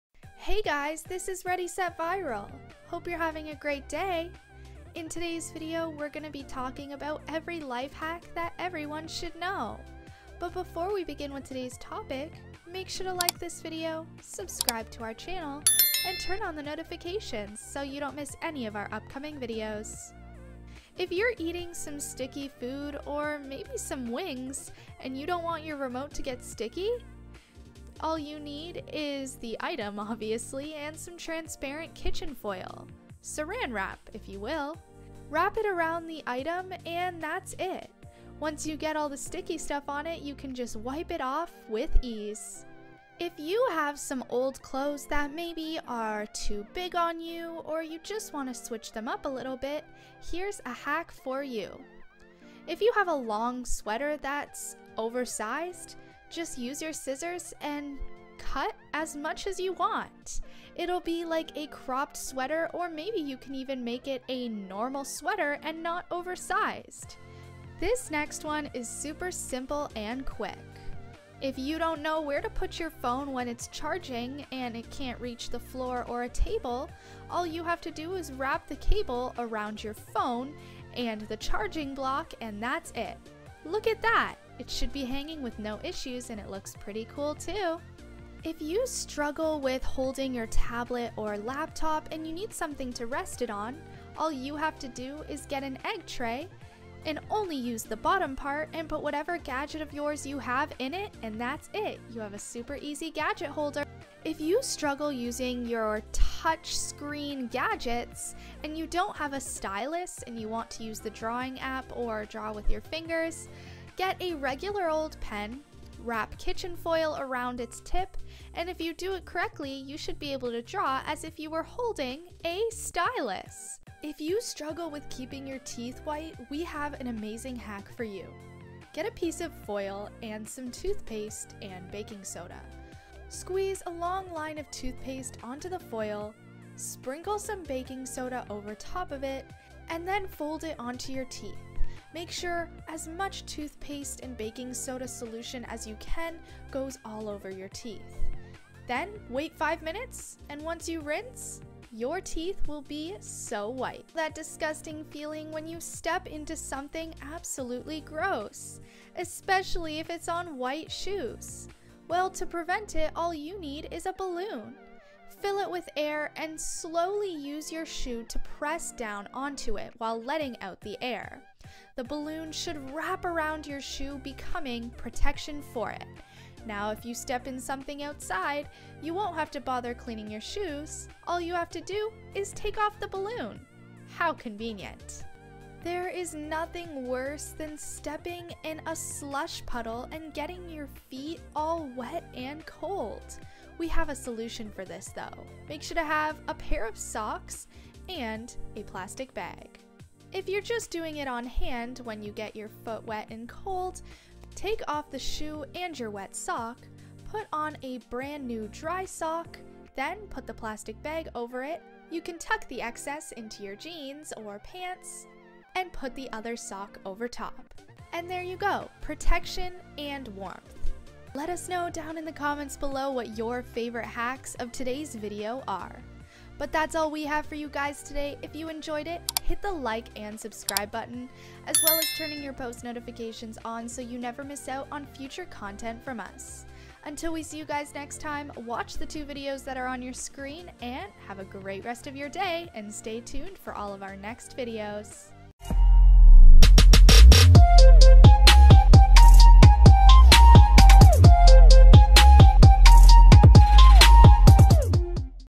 Every Life Hack That EVERYONE Should Know!
Hey Ready Set Viral Fans!  Check out this amazing video where we show you life hacks that everyone should know!

0:00 Intro
0:28 How to prevent a sticky remote control
0:43 How to reuse an oversized sweater
1:07 How to charge a phone without a table nearby
1:28 Tablet, Laptop, or iPad holder (egg carton)
1:44 How to make a stylus with foil
2:05 How to whiten your teeth
2:34 How to prevent shoes from getting dirty
3:05 How to prevent socks from getting wet